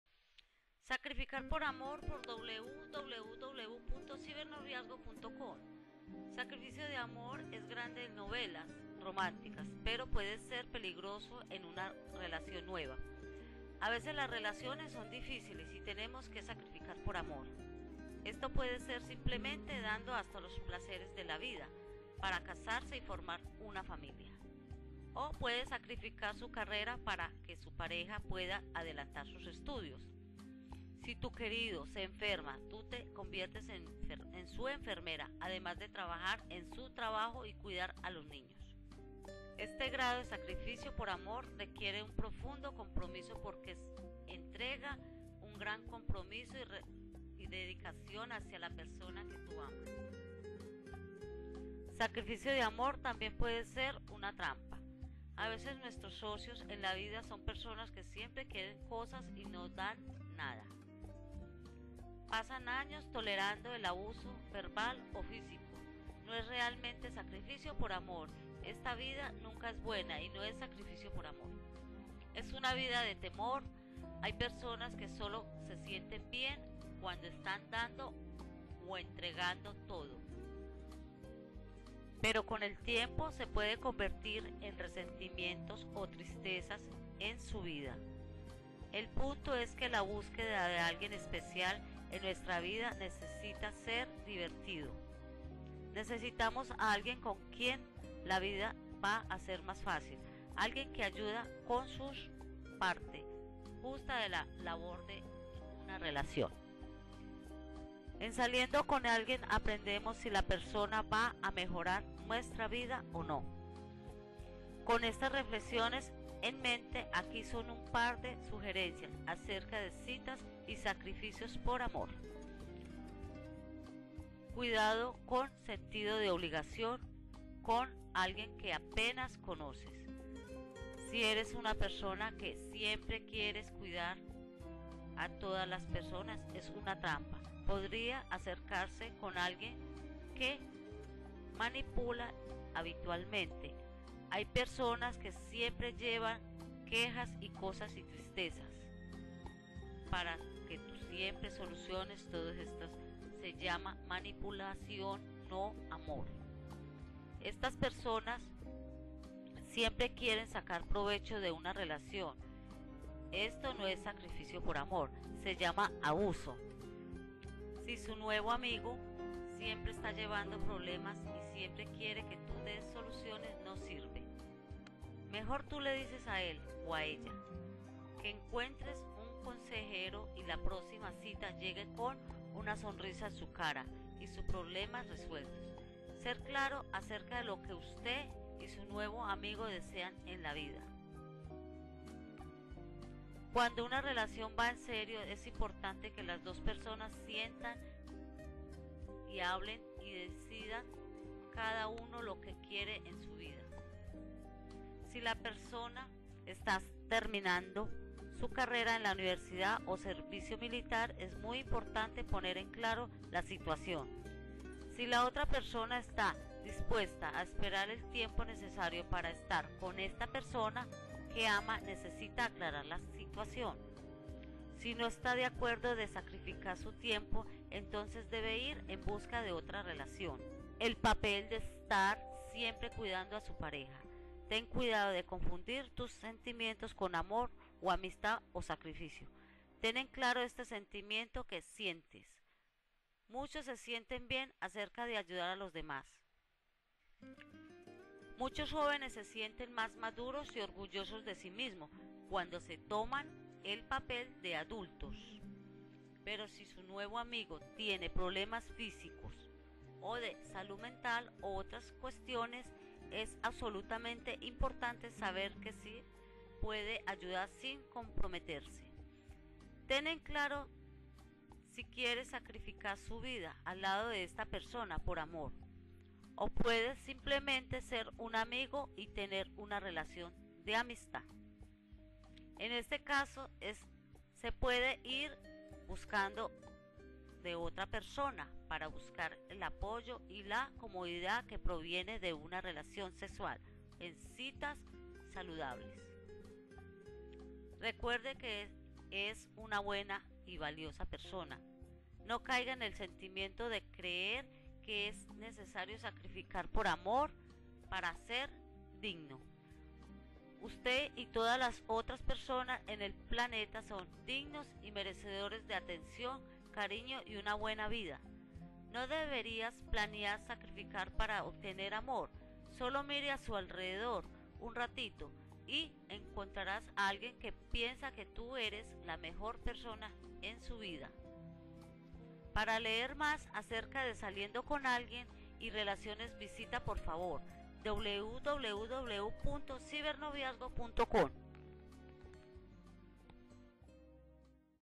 Sacrificar por amor
Sacrificio de amor es grande en novelas románticas, pero puede ser peligroso en una relación nueva.
A veces las relaciones son difíciles y tenemos que sacrificar por amor.

Esto puede ser simplemente dando hasta los placeres de la vida para casarse y formar una familia.

O puede sacrificar su carrera para que su pareja pueda adelantar sus estudios.

Si tu querido se enferma tú te conviertes en su enfermera además de trabajar en su trabajo y cuidar a los niños.
End of 1
Este grado de sacrificio por amor requiere un profundo compromiso. Porque es entrega un gran compromiso y dedicación hacia la persona que tu ama.

Sacrificio de amor también puede ser una trampa.

A veces nuestros socios en la vida son personas que siempre quieren cosas y no dan nada.

Pasan años tolerando el abuso verbal o físico no es realmente sacrificio por amor. Esta vida nunca es buena y no es sacrificio por amor.

Es una vida de temor. Hay personas que sólo se sienten bien cuando están dando o entregando todo.
End of 2
Pero con el tiempo se puede convertir en resentimientos o tristezas en su vida.

El punto es que la búsqueda de alguien especial en nuestras vidas necesitas ser divertido.

Necesitamos a alguien con quien la vida va a ser más fácil, alguien que ayuda con su parte justa de la labor de una relación.

En saliendo con alguien aprendemos si la persona va a mejorar nuestra vida o no.

Con estas reflexiones en mente aquí son un par de sugerencias acerca de citas y sacrificar por amor.
Cuidado con un sentido de obligación con alguien que apenas conoces
Si eres una persona que siempre quiere cuidar a todas las personas es una trampa. Podría acercarse con alguien que manipula habitualmente.
Hay personas que siempre llevan quejas y cosas tristes para que tú siempre soluciones todo. Esto se llama manipulación, no amor.
Estas personas siempre quieren sacar provecho de una relación.
Esto no es sacrificio por amor.
Se llama abuso.

End of 3
Si su nuevo amigo siempre está llevando problemas y siempre quiere que tú des soluciones no sirve.
Mejor tú le dices a él o ella que encuentres un consejero y la próxima cita llegues con una sonrisa en su cara y sus problemas resueltos.
Ser claro acerca de lo que usted y su nuevo amigo desean en la vida
Cuando una relación va en serio es importante que las dos personas sienten y hablen y decir cada uno lo que quiere en su vida.
Si la persona está terminando su carrera en la universidad o servicio militar es muy importante poner en claro la situación.
Si la otra persona está dispuesta esperar el tiempo necesario para estar con esta persona que ama necesita aclarar la situación.
Y si no está acuerdo de sacrificar su tiempo entonces debe ir en busca de otra relación.
El papel de estar siempre cuidando a su pareja
Ten cuidado de confundir tus sentimientos con amor o amistad o sacrificio.
Ten en claro este sentimiento que sientes.
Muchos se sienten bien acerca de ayudar a los demás.
End of 4
Muchos jóvenes se sienten más maduros y orgullosos de sí mismos cuando se toman el papel de adulto.
Pero, si su nuevo amigo tiene problemas físicas o de salud mental y otras cuestiones es absolutamente importante saber que puede ayudar sin comprometerse.
Ten en claro si quiere sacrificar su vida al lado de esta persona por amor. O puedes simplemente ser un amigo y tener una relación de amistad.
En este caso se puede ir en busca de otra persona para buscar el apoyo y la comodidad que proviene de una relación sexual y citas saludables.

Recuerde que es una buena y valiosa persona
No caiga en el sentimiento de creer que es necesario sacrificar por amor para ser digno.
Usted y todas las otras personas en el planeta son dignos y merecedores de atención, cariño y una buena vida.
No debería planear sacrificar para obtener amor. Sólo mire a su alrededor un ratito y encontraras a alguien que piensa que tú eres la mejor persona en su vida.
Para leer más acerca de saliendo con alguien y relaciones visita por favor